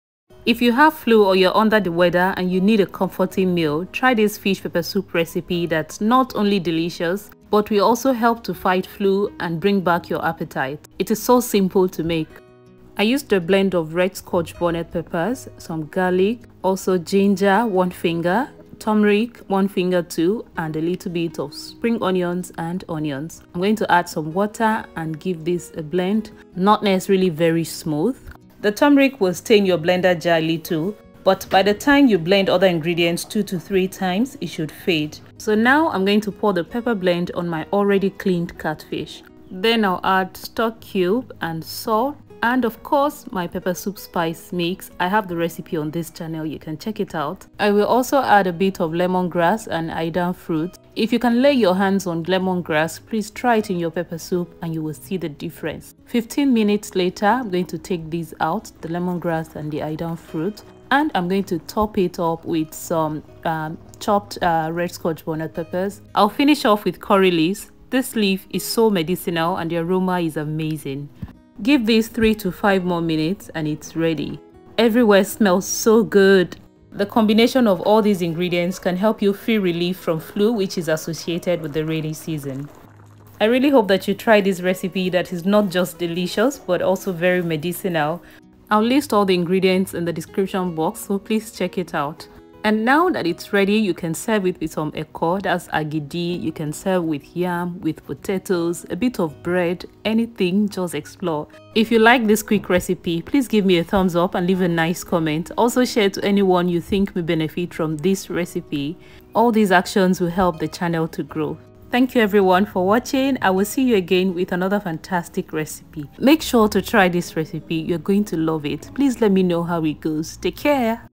Fight Flu And Restore Your Appetite With This Medicinal Catfish Peppersoup Recipe
Flu fighting, appetite restoring deliciousness Catfish peppersoup recipe

Ingredients
Catfish
Red scotch bonnet peppers
Turmeric
Ginger
Garlic
Onion
Spring onion
Lemon grass
Aidan fruit
Stock cube
Salt

Peppersoup spice mix recipe👇🏽